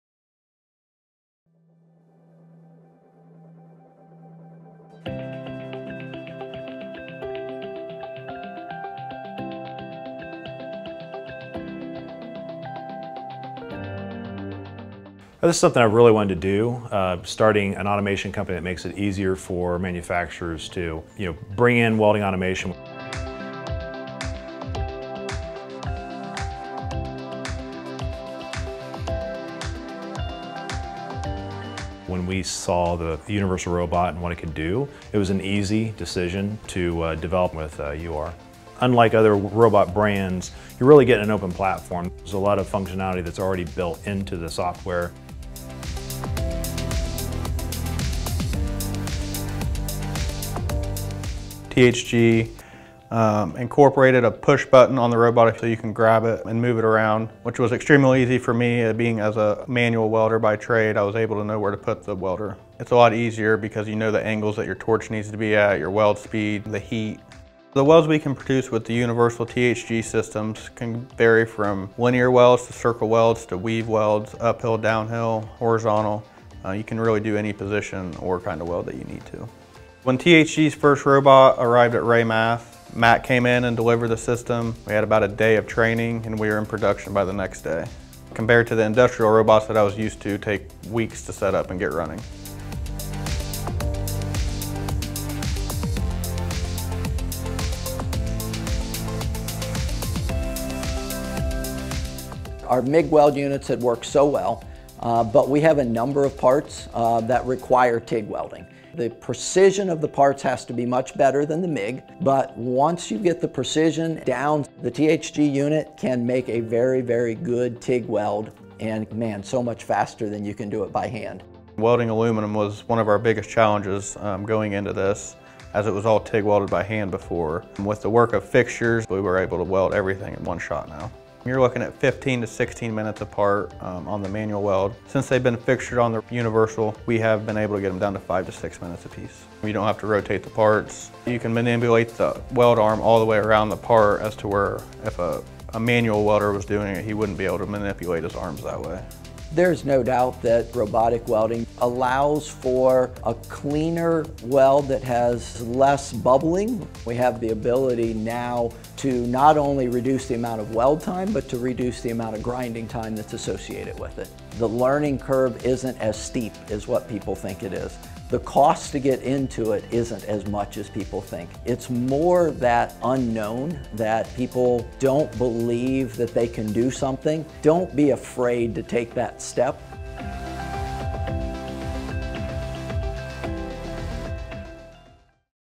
From MIG to TIG Welding with Cobots: Raymath's Application Journey with THG Automation
Complex aluminum welding was one of sheet metal fabricator Raymath’s biggest challenges for growth; it was all TIG welded by hand with precise welds on top and underneath, with difficult angles and heat settings, and multiple moving parts. After seeing success in MIG welding with THG Automation's UR-cobot powered welding solutions, Raymath approached THG for a TIG solution.